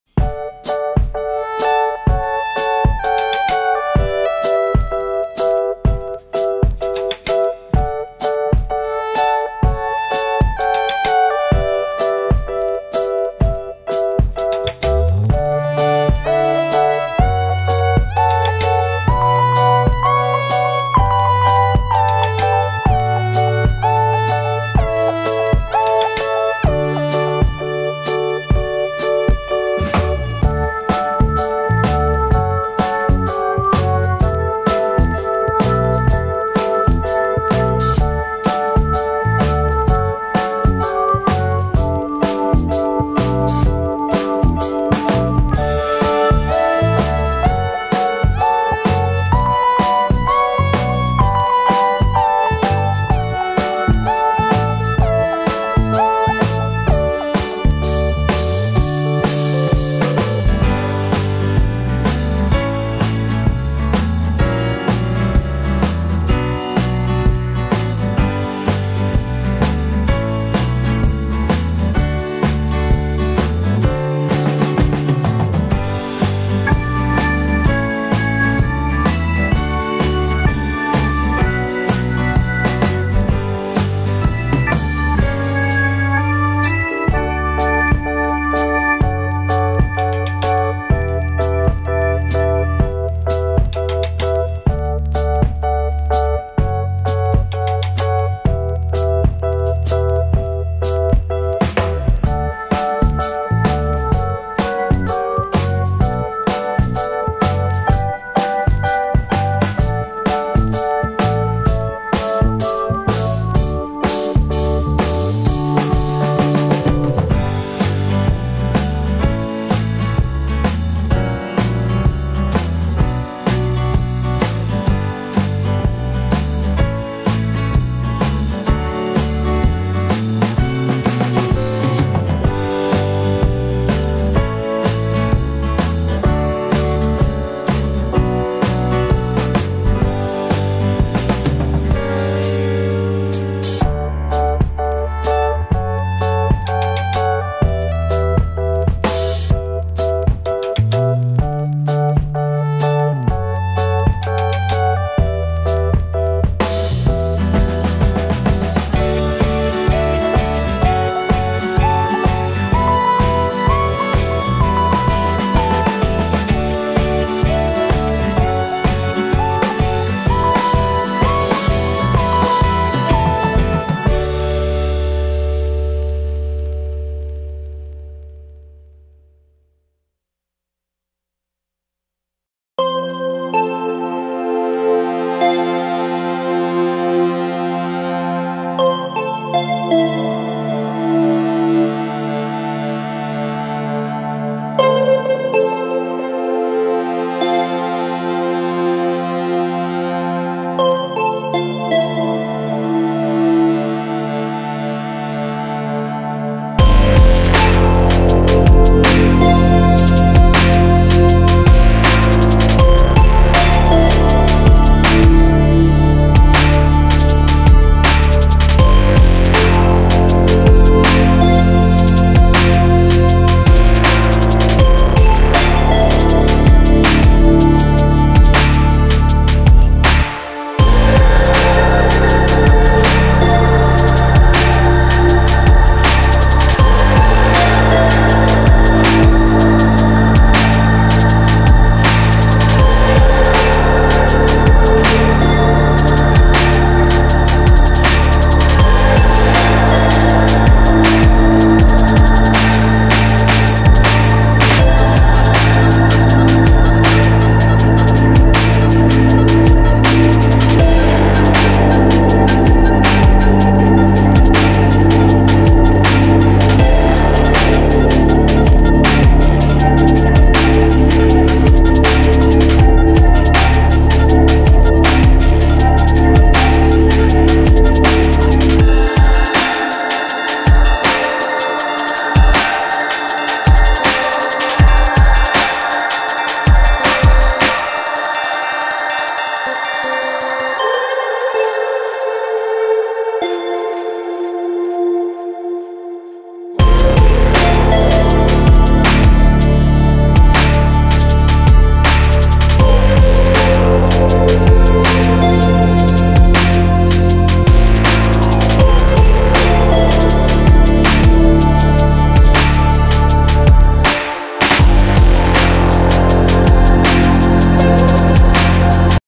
Gonitwa Łososiowa 2015- Bardy
Wyciąganie z wody uczestników Gonitwy- most Bardy.